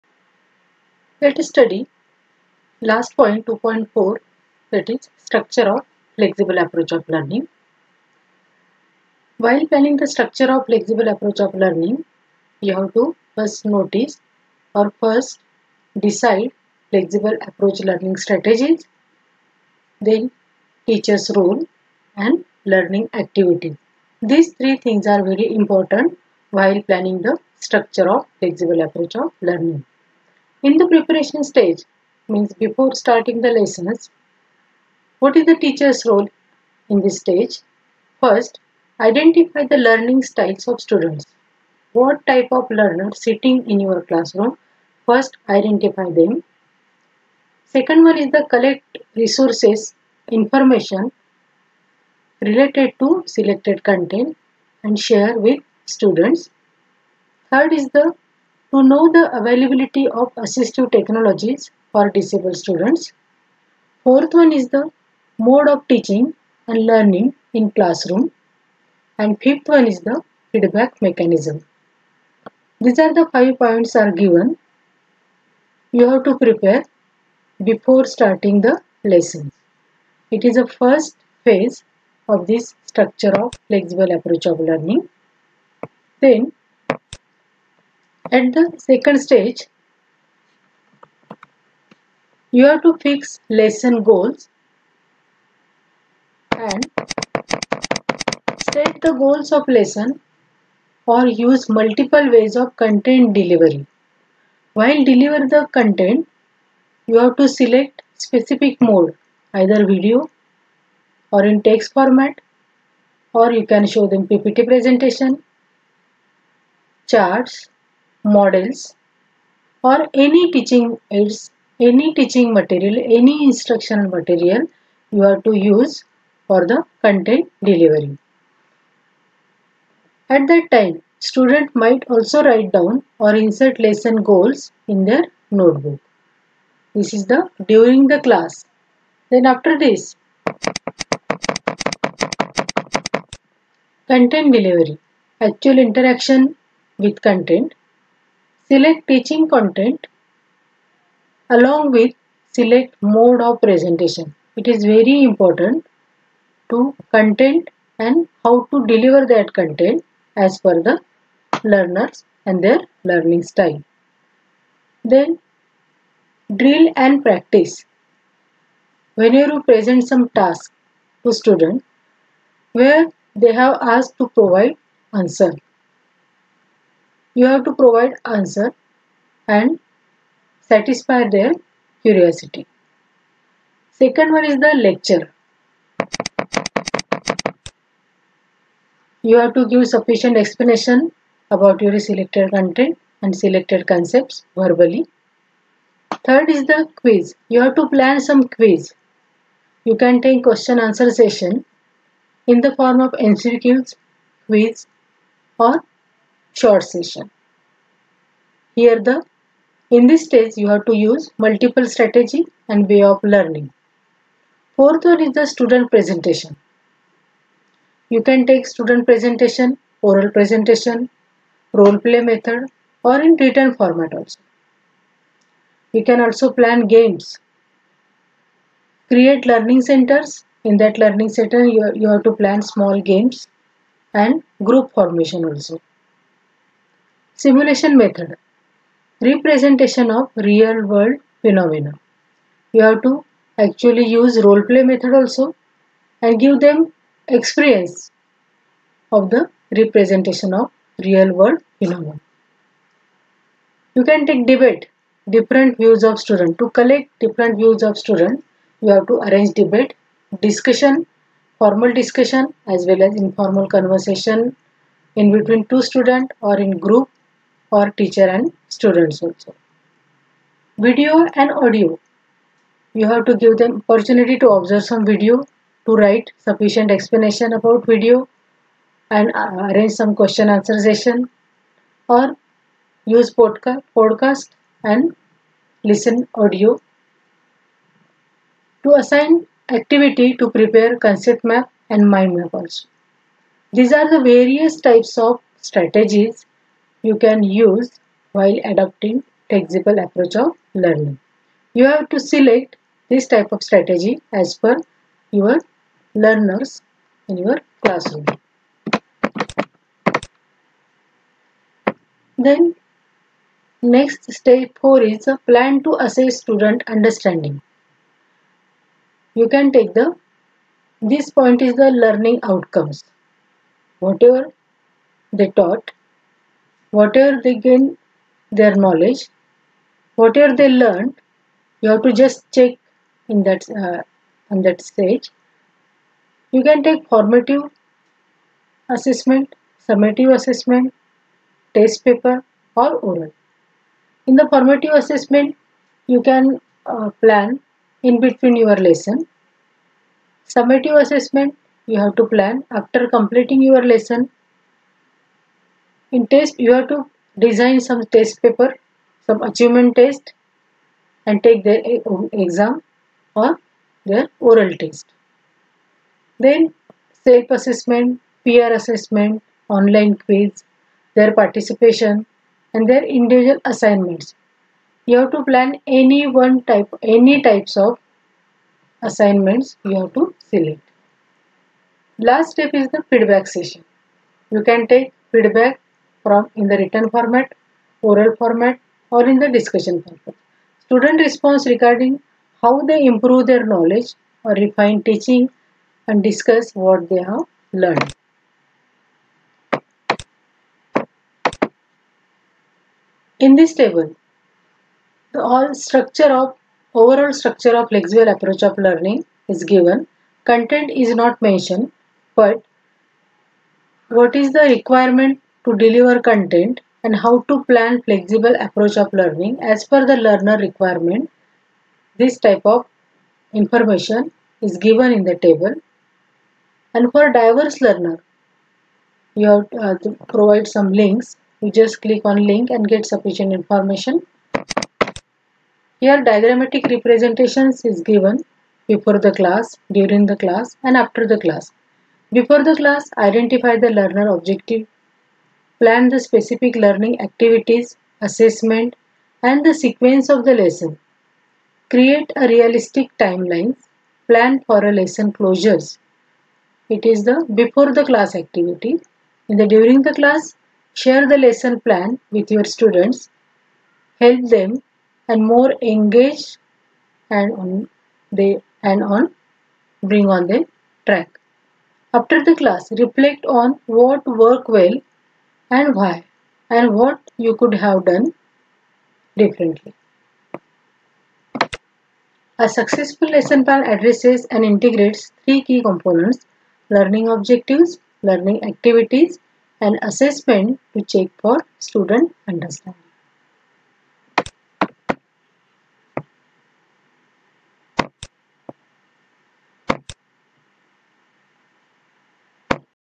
Structure of FAL
In this video how to plan and adopt flexible method in classroom